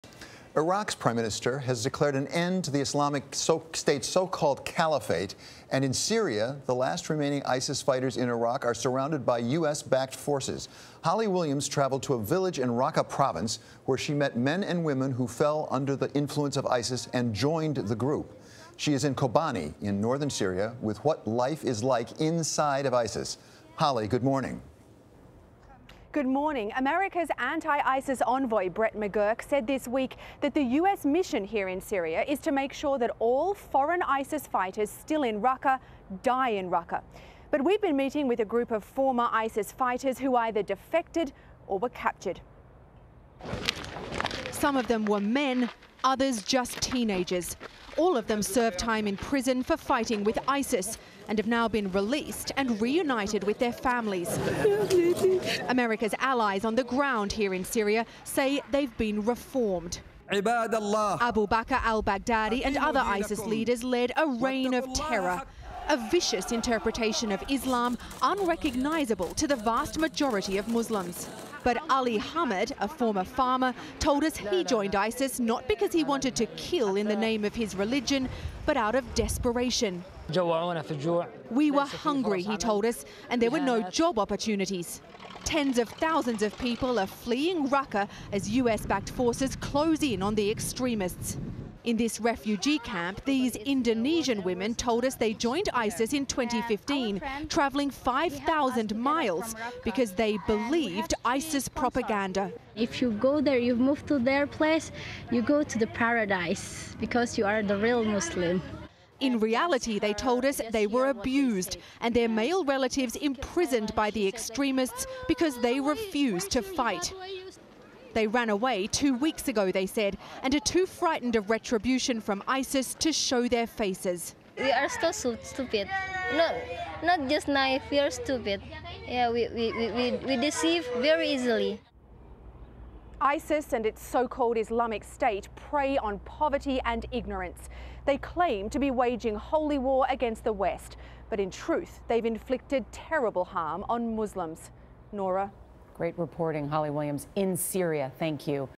Former ISIS members reveal why they joined terror group
The last remaining ISIS fighters in its self-proclaimed capital of Raqqa, Syria are surrounded by U.S.-backed forces. Holly Williams met with a group of former ISIS fighters who either defected or were captured. One man told her he joined ISIS not because he wanted to kill in the name of his religion - but out of desperation.